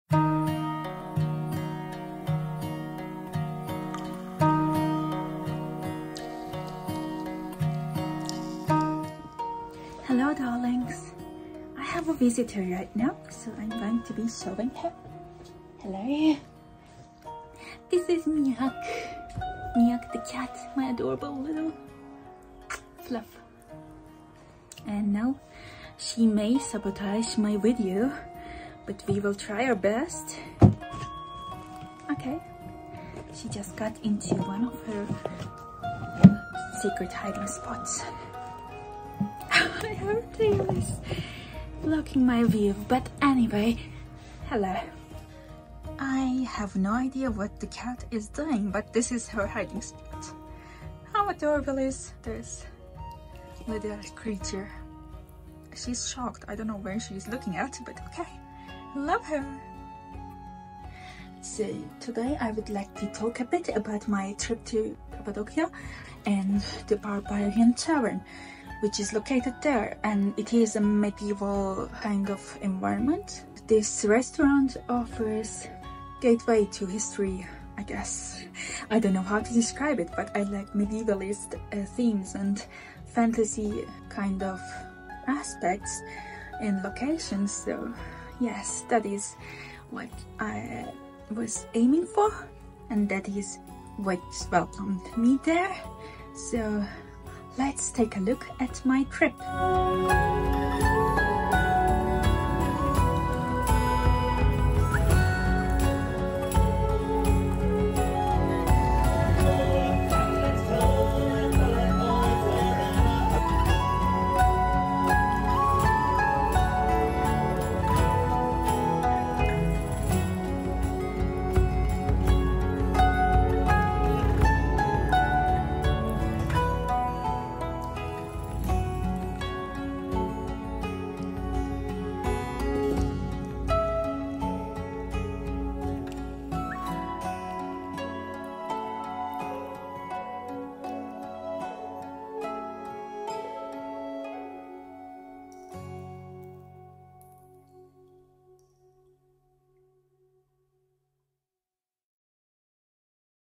Barbarian Medieval Tavern | VLOG
Welcome traveller to this cosy tavern; sit by the hearth if you wish to. Now, do tell me what you would like to have: Something warm and delicious to fill your belly, or simply a drink?

✶ Credits
• Shot and edited on iPhone